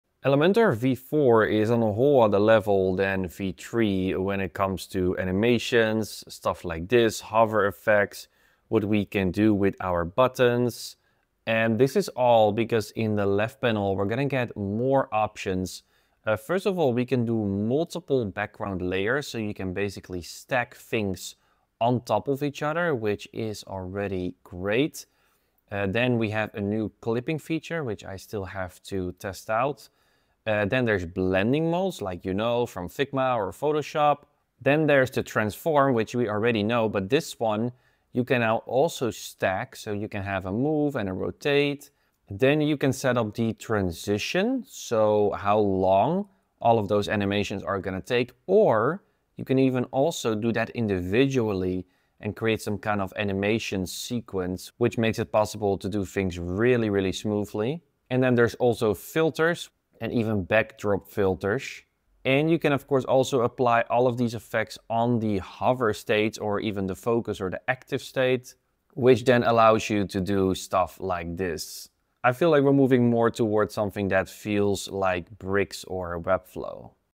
Hover effects in Elementor V4 are so nice!
The hover animations in Elementor V4 are on another level, because we do get a lot more controls. Not just presets.